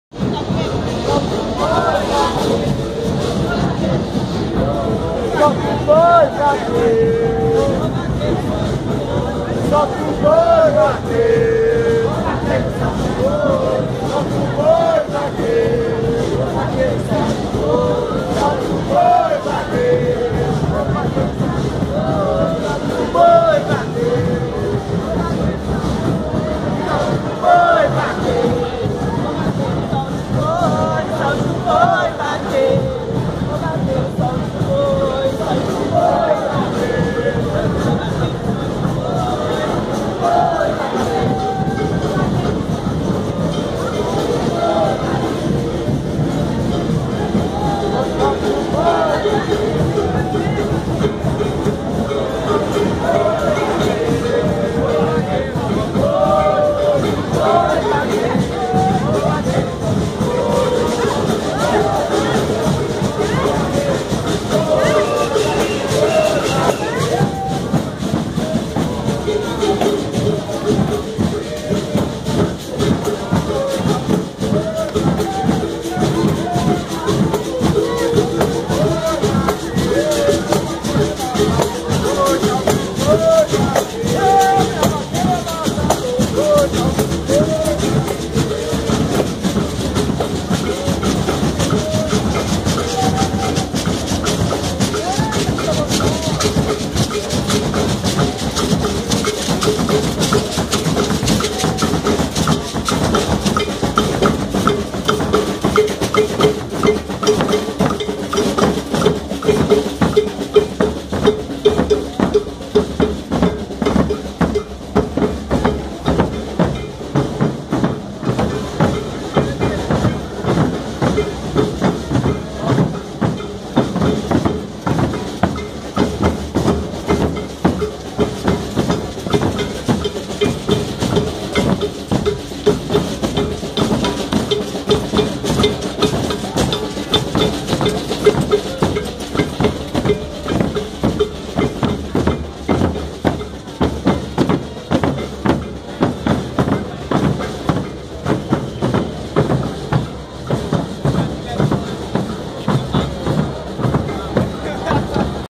Desfile do Boi Multicor - Carnaval 2016
A Boiada Multicor no carnaval de 2016, trazendo a importância da multiculturalidade, pluralidade e respeito a ancestralidade africana.